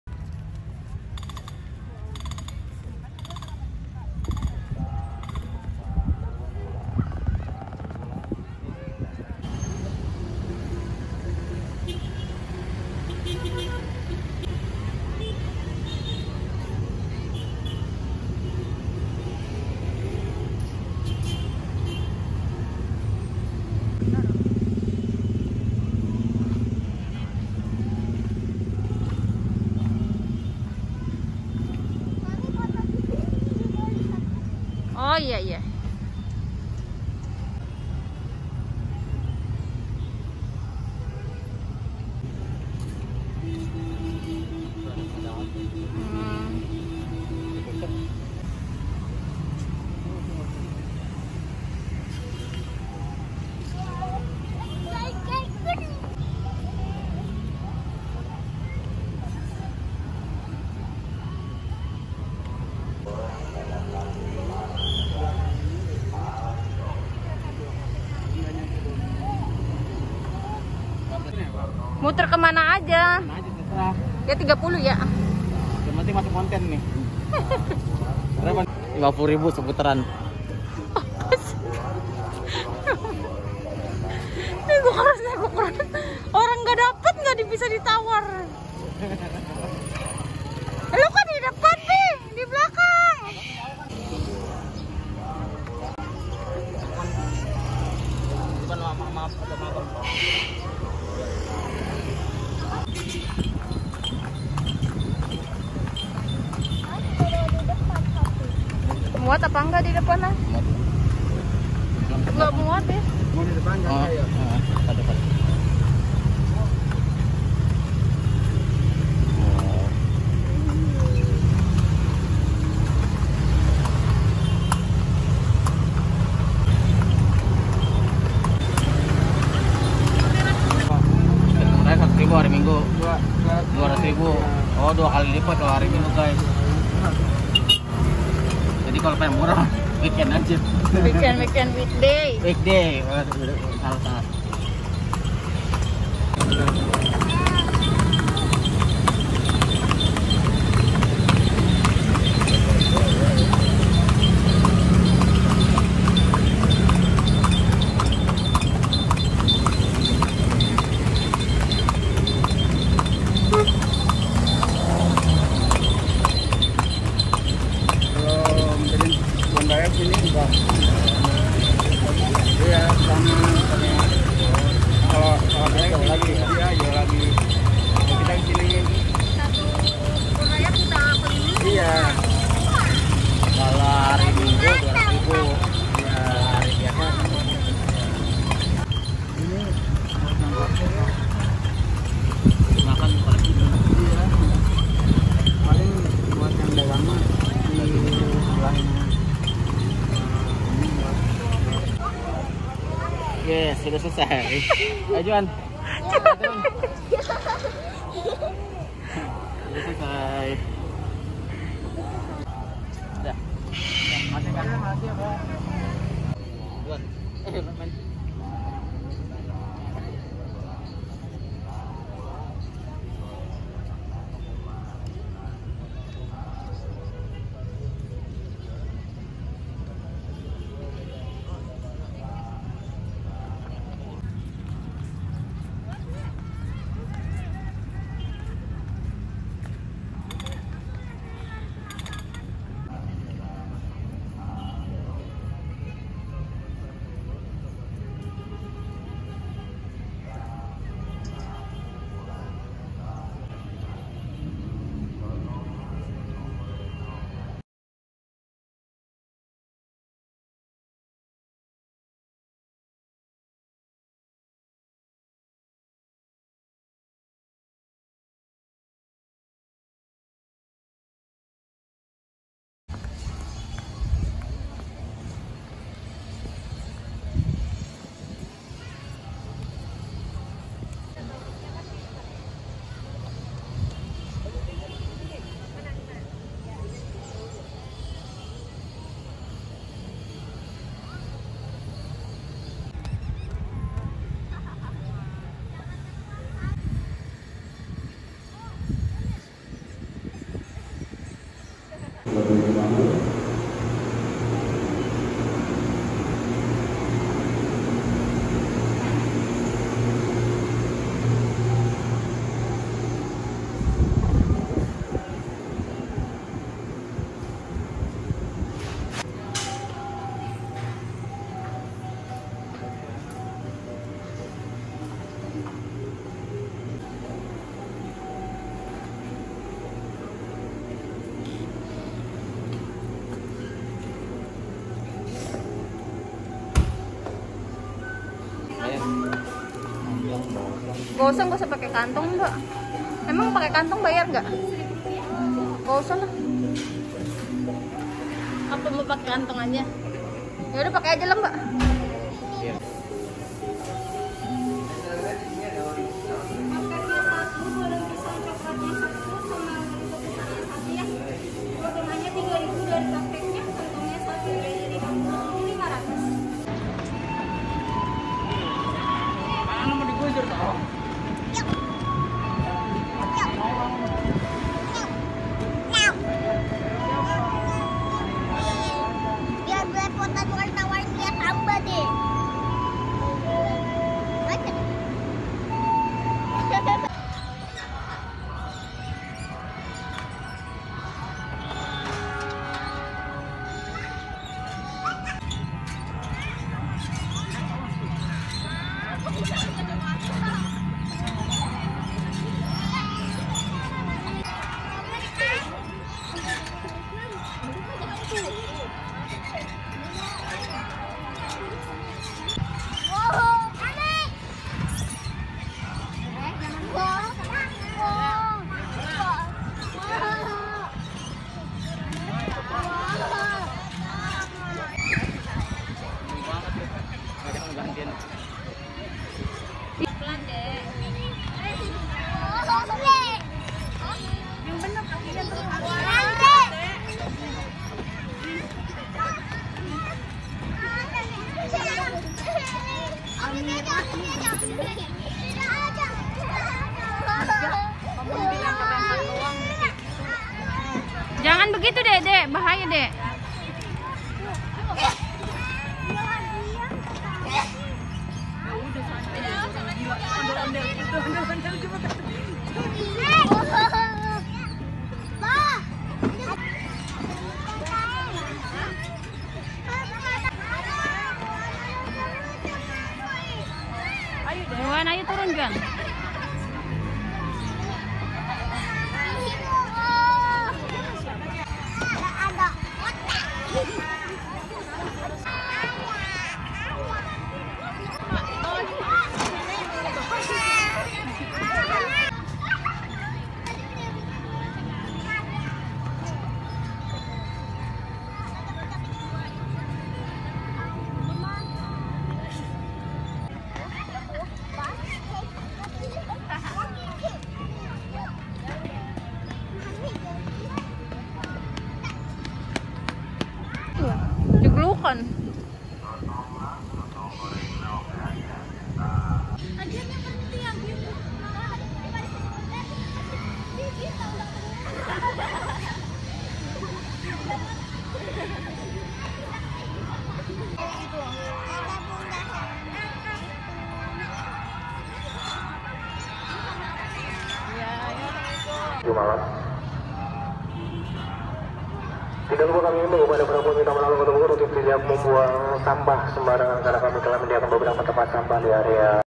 Bermain dan Naik Delman di Alun-Alun Kota Bogor
Berwisata ke alun-alun kota Bogor kita bermain perosotan, ayunan, bermacam - macam alat olah raga (sebenarnya anak-anak tidak diperbolehkan) yang paling seru bisa berlari keseluruhan area alun-alun yg luas dan indah berbagai fasilitas yg mendukung menjadikan alun-alun kota Bogor bisa kita jadikan tujuan wisata keluarga, terimakasih,